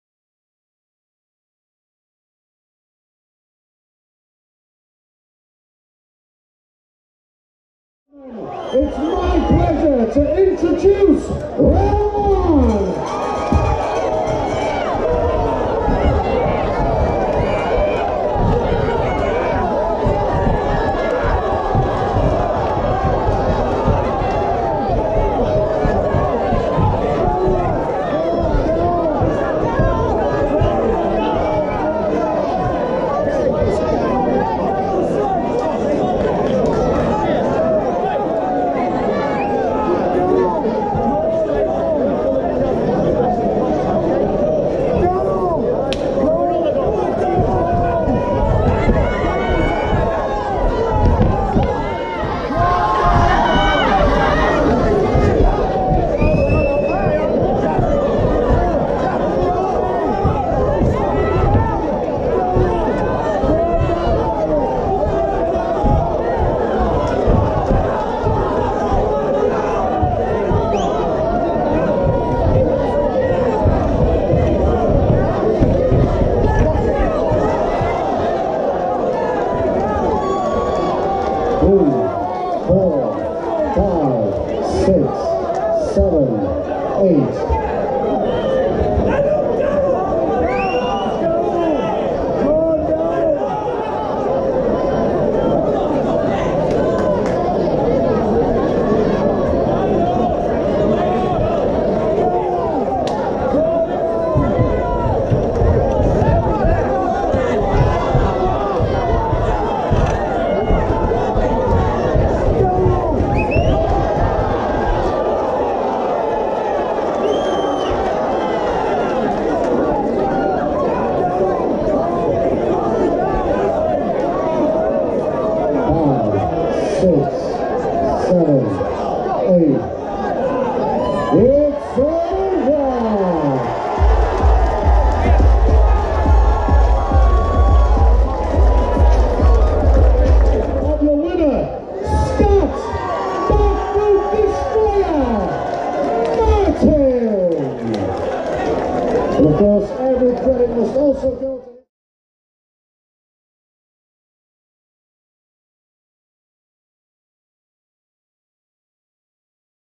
Ultra White Collar Boxing | Chesterfield | Darryl Hodgson
Chesterfield Ultra White Collar Boxing 24th November 2018

8 Weeks FREE training / Get in the best shape of your life/ Raise money for Cancer Research UK